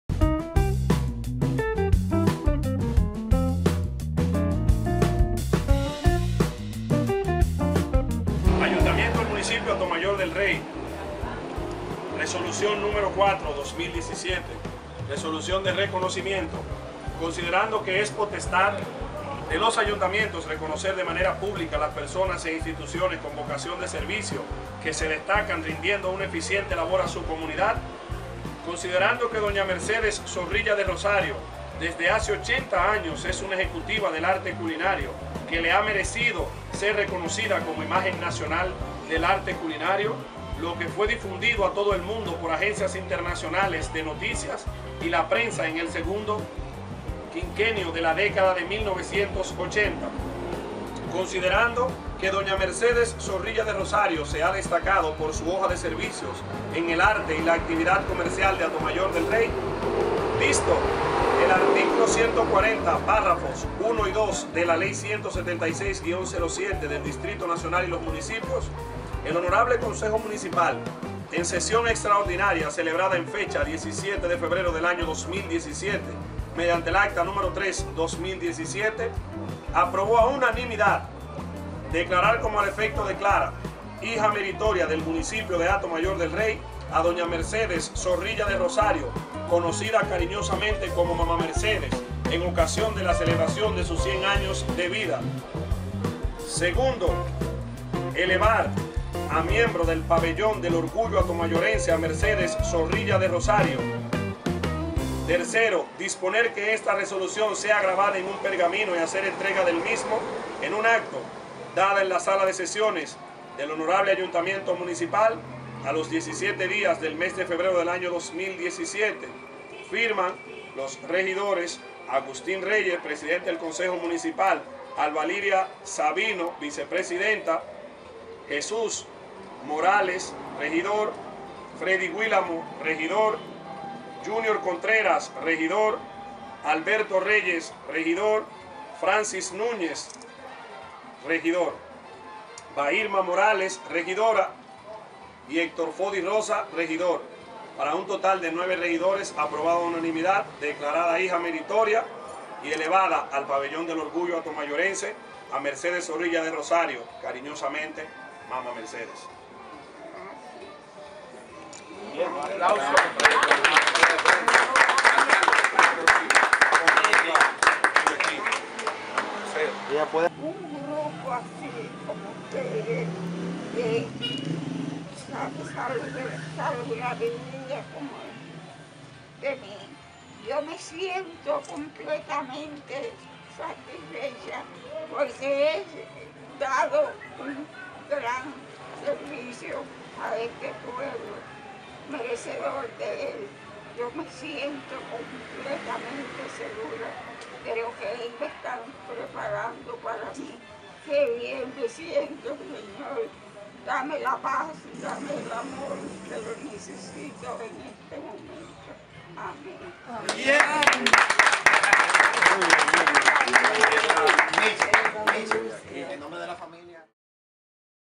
Reconocimiento a Mercedes Zorrilla de Rosario "Mama Mercedes"
Honor a quien Honor Merece! Reconocimiento Declarando Hija Meritoria y elevando al Pabellón del Orgullo Hatomayorense a Mercedes Zorrilla de Rosario "Mama Mercedes" una honesta mujer que nació el 12 de febrero de 1917 y que por su arte culinario fue seleccionada “Imagen Nacional”, lo que fue difundida por todo el mundo por agencias internacionales de noticias y la prensa nacional en el segundo quinquenio de la década de 1980. Su entrega al arte culinario, que con tanto amor y ahínco ejerció en el siglo XX en Hato Mayor, le ha servido para que a diario reciba las visitas de reposteras y jóvenes que buscan conocer de recetas para aprender a cocinar.